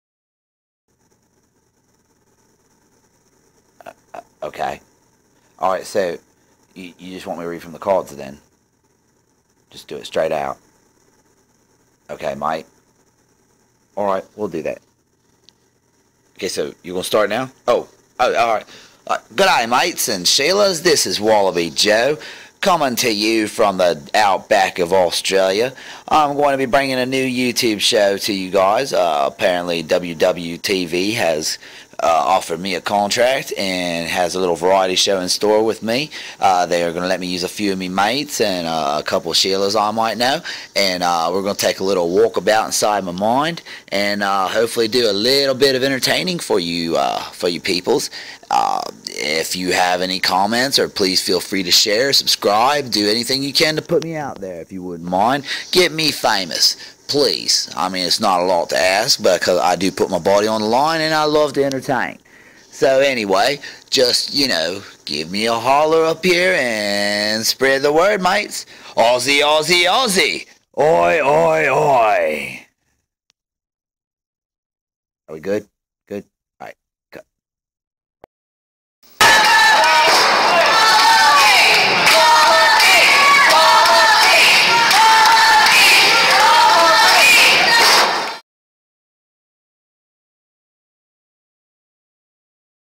Wallabee & Associates: The Show Promotion 1
WWTVproductions aquired professional wrestler Wallabee Joe and his associates, such as his tag team partner Rassle Crowe, in a variety show venture in attempts at entertaining the world.